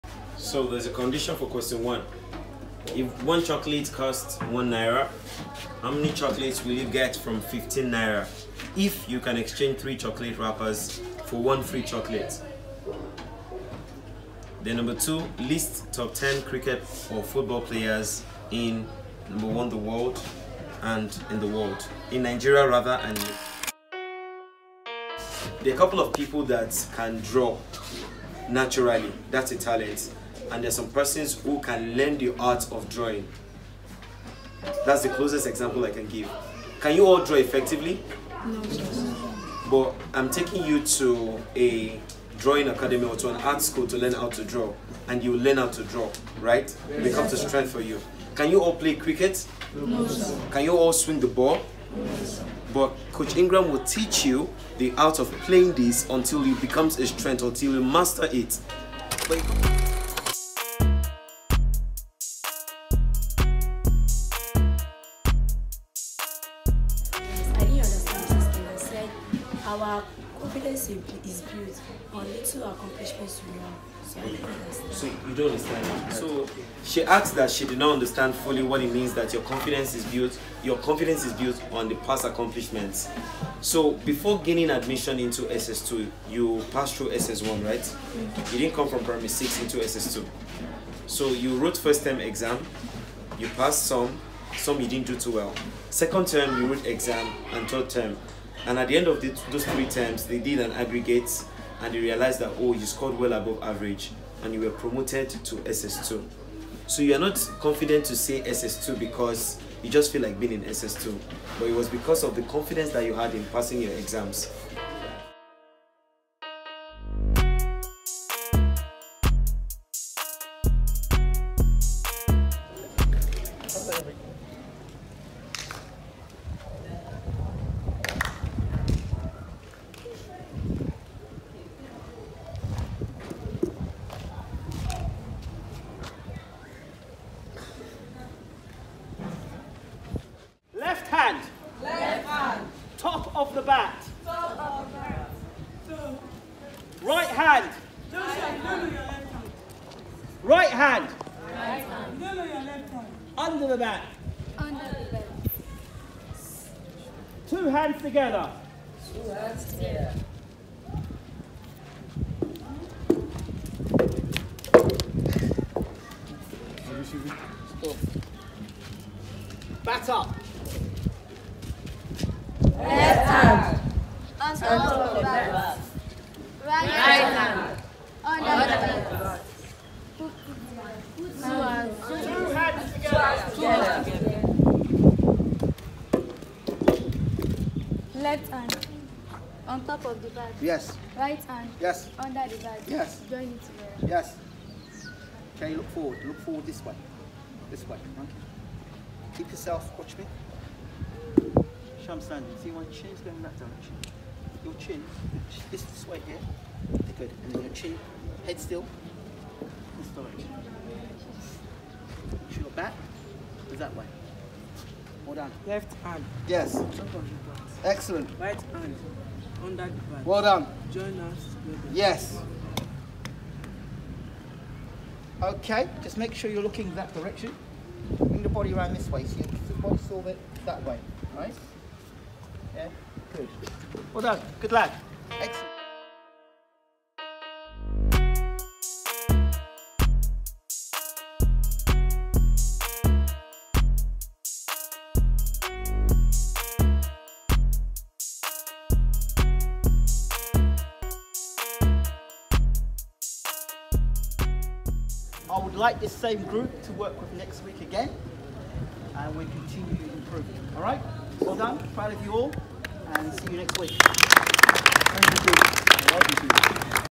Mighty Pillars School discuss self confidence in class and begin cricket training!
The RCIP Ikorodu Brighter Futures Program continues at Mighty Pillars School in Agric, Ikorodu with Neem Foundation Mr Olajide facilitating the classroom discussions whilst Project Lead Ingram Jones oversees the cricket development programs across Ikorodu.

The Brighter Futures Program is a crime intervention program dedicated to build character resilence and raise awareness of the dangers of cybercrime and that there are other ways to live a successful progressive life by making better life choices but it starts now.